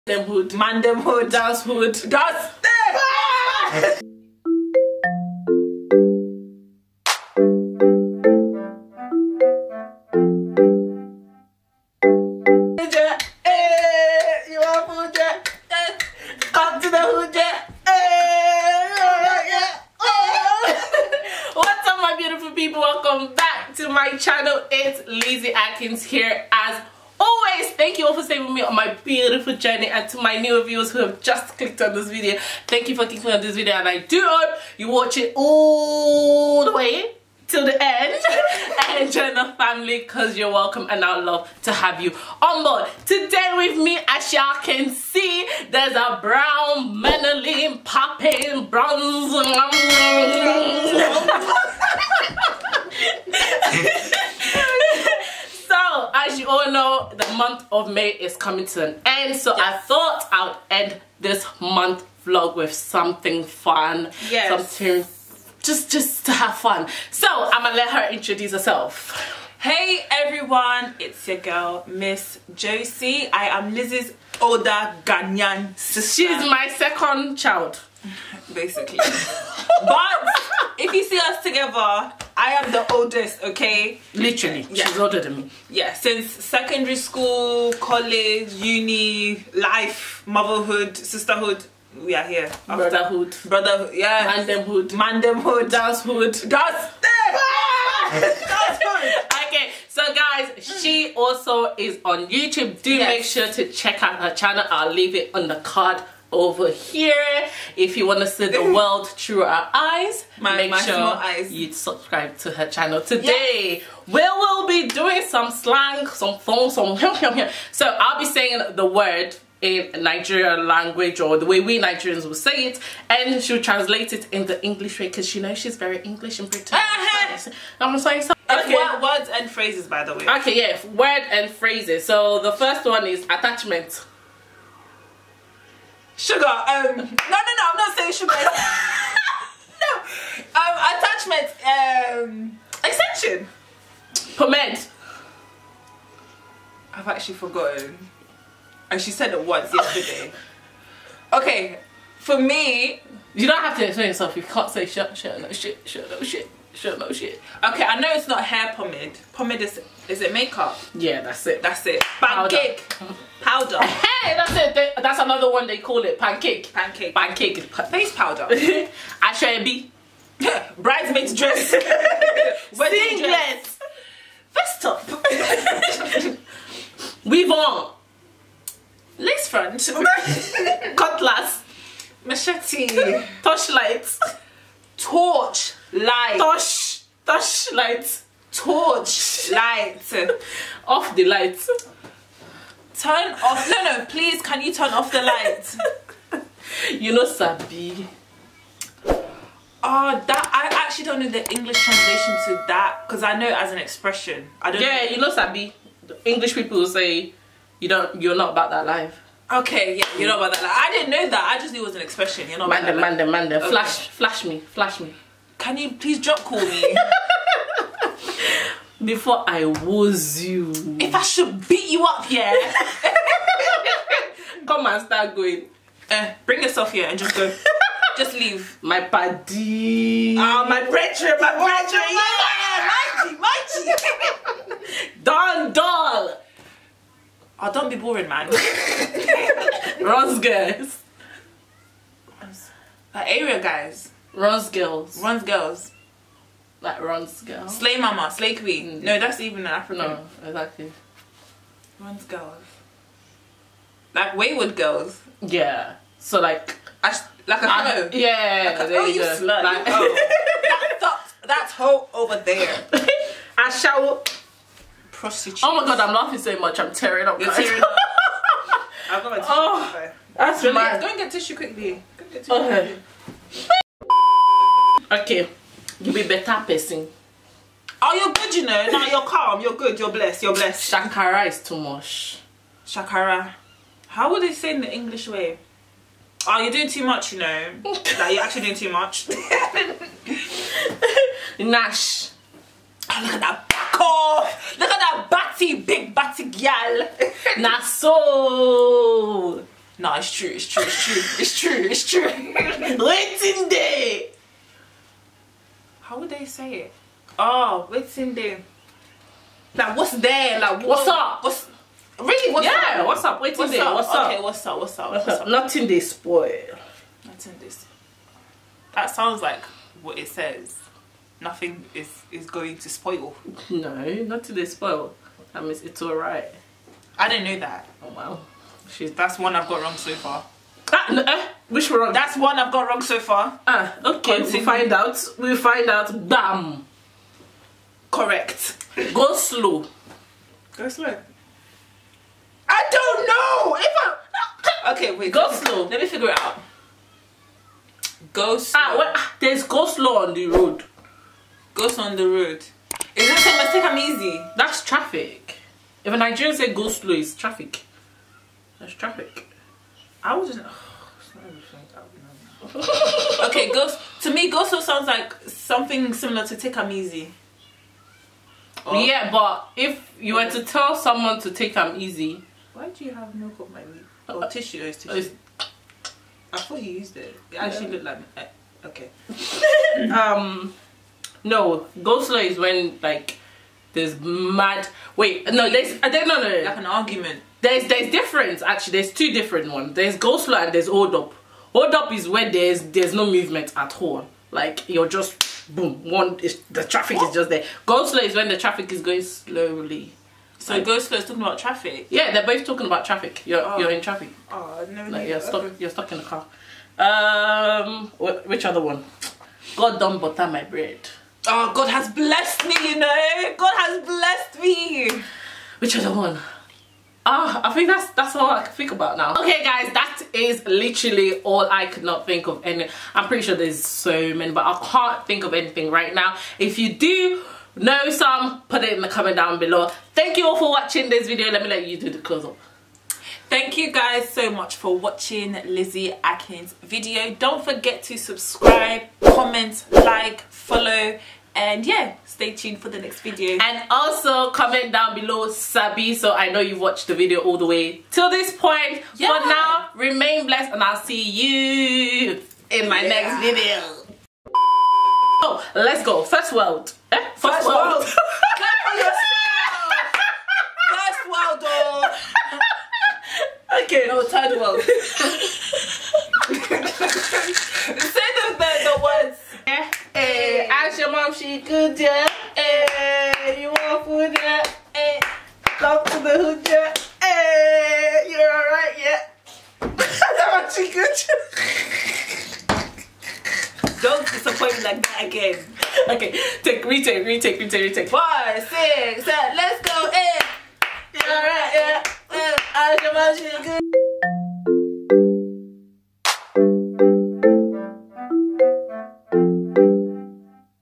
Accent Challenge-- Nigeria vs British// Common Nigerian PHRASES/SLANG. Pidgin English
Whats up my beautiful people, we are on day 29 now, coming closer to the end of malog. it has been a very difficult, challenging yet fun month of creativity. I hope you all love each video. Todays' video is a collaboration with MISS JOSIE. we had some much fun filming tis video, watch the video to share in out fun.

Thank you all for joining in and staying with me.